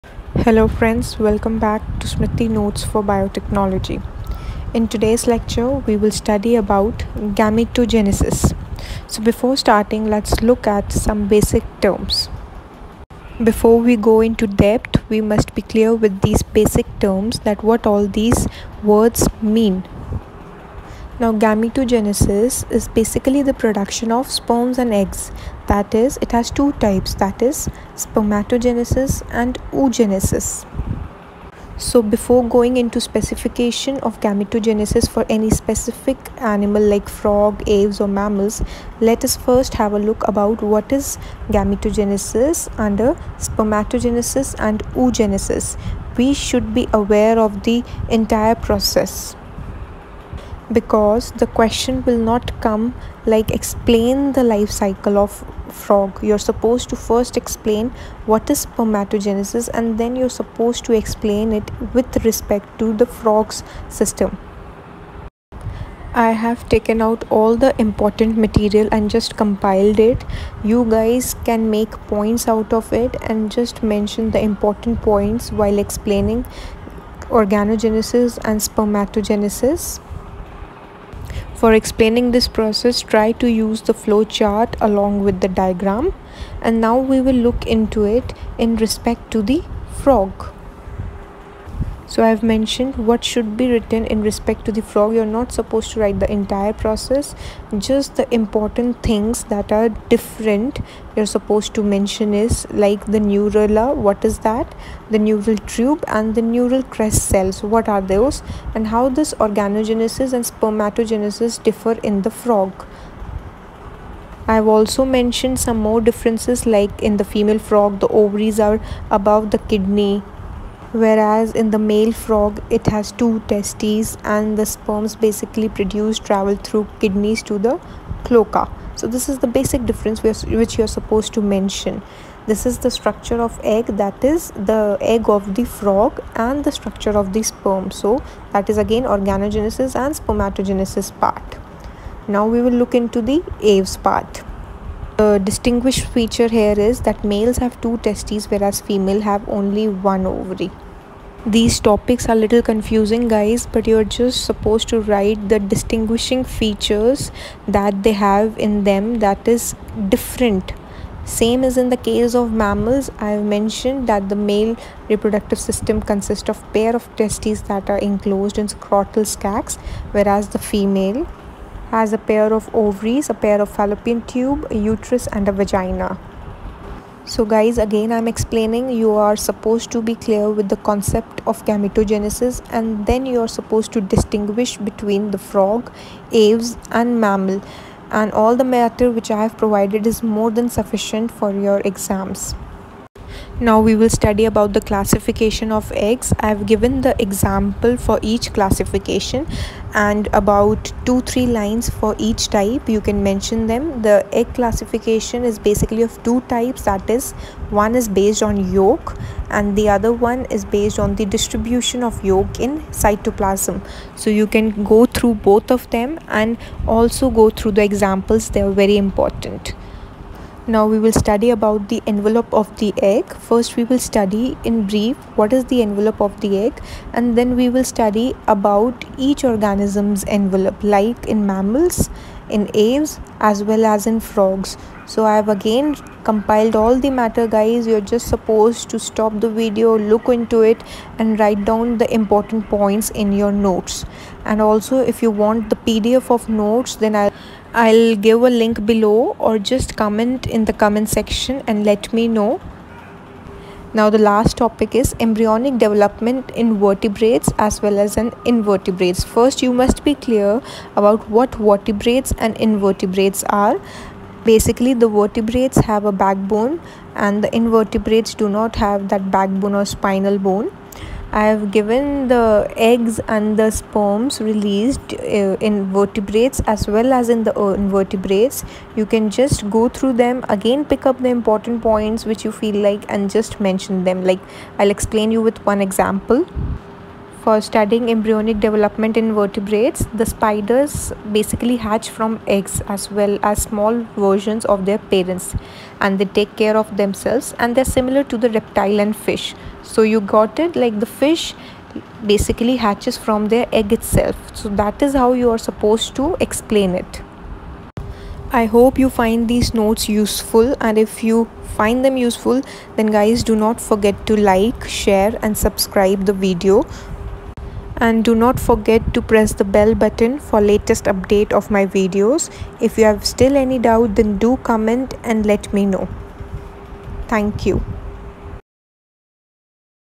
Organogenesis Spermatogenesis- FROG , AVES , MAMMALS , Egg Classification | Egg Envelope
Hello everyone ,
In this video we will study about Gametogenesis thaat is fusion of gametes.
It is of two types that is Organogenesis and Spermatogenesis .
We will study about FROG , AVES , MAMMALS .
First our basic concept should be clear then we can mention the distinguishing feature about frogs , aves and mammals .
We will also study about the classificaton of eggs that is of two types .
egg envelope structure and about the invertebrate and vertebrate eggs and sperms .
 These topics are difficult to get so i have compiled the whole matter you guys can use the material .
 Also if you want the pdf of these notes then do comment and let me know ..

amity_notes_